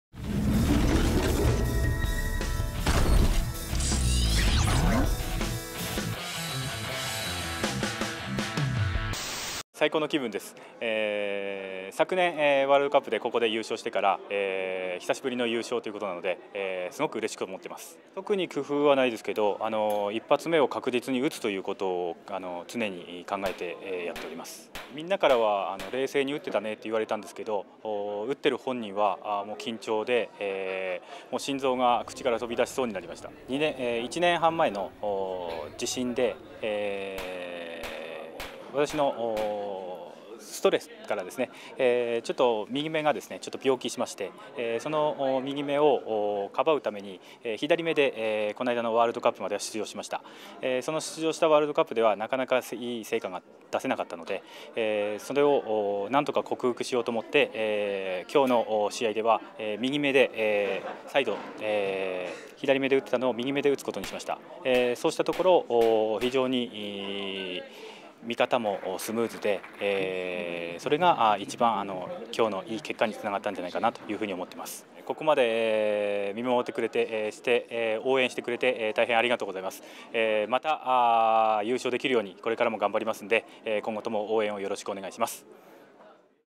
Interview of Teruyoshi Akiyama - Munich 2013 ISSF World Cup
Interview of Teruyoshi Akiyama - 25m Men's Rapid Fire Pistol gold medalist at the Munich 2013 ISSF Rifle and Pistol World Cup.

PODIUM:
1. AKIYAMA Teruyoshi
2. ZHANG Fusheng
3. BONDARUK Roman

China's Zhang Fusheng declares love to his team ("Like a family") right after winning his first medal ever at the Rapide Fire Pistol event in Munich. The 19-year-old shooter now wants to catch up with Yang Haoran, his winning teammate (17-year-old and 2-time ISSF Gold medallist).